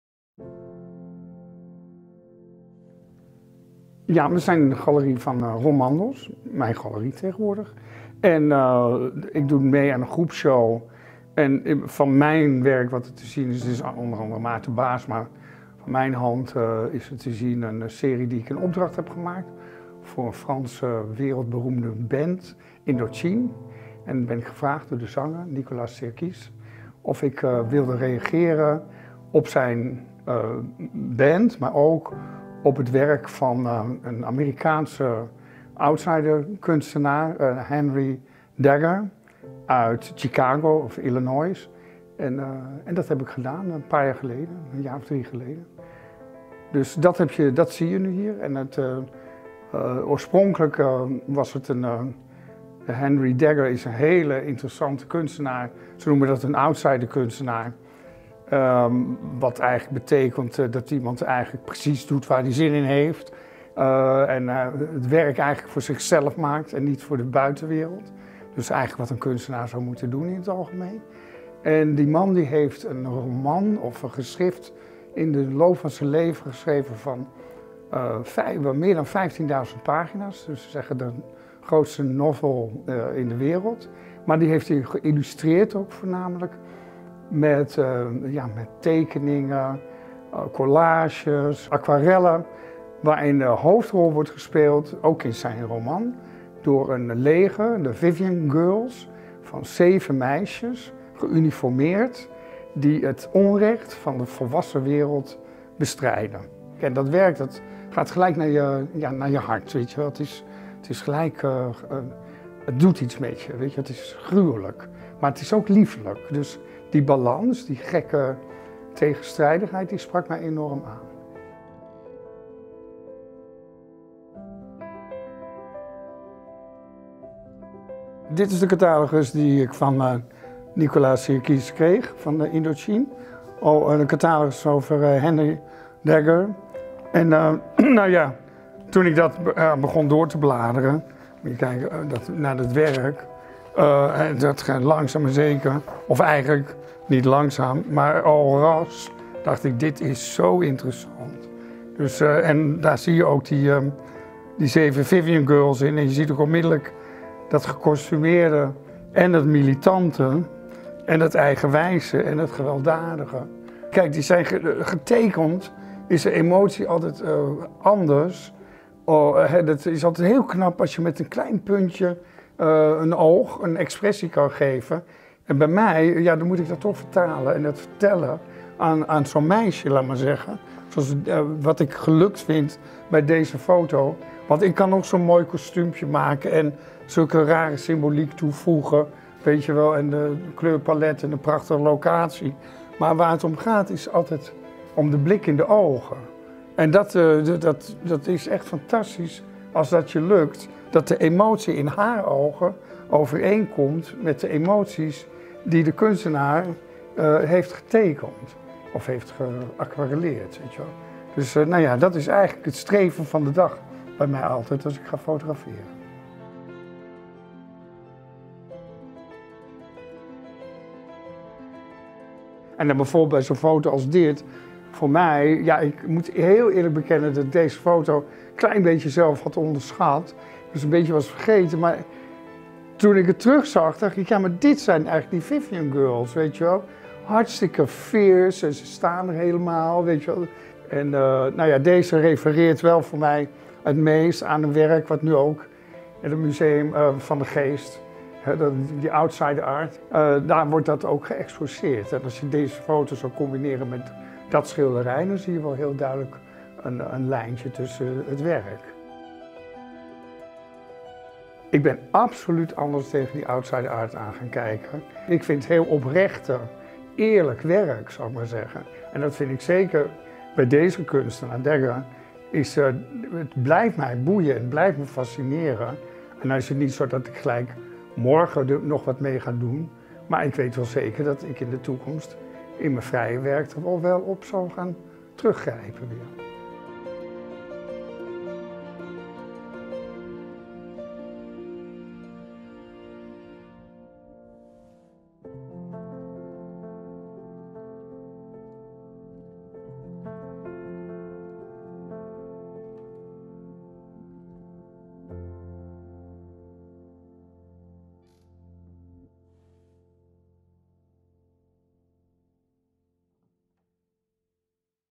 Erwin Olaf X Museum van de Geest
In 2017 kwam fotograaf Erwin Olaf in contact met de aquarellen van de Amerikaanse outsider kunstenaar Henry Darger. Hij raakte geïnspireerd door Darger’s kleurrijke tekeningen van de Vivian Girls en creëerde de portrettenserie ‘Indochine’ voor het nieuwe album van een Franse New Wave band.

De serie was recent te zien in Galerie Ron Mandos. De werken van Henry Darger zijn t/m 5 september 2021 te zien in Museum van de Geest – Outsider Art, in Amsterdam.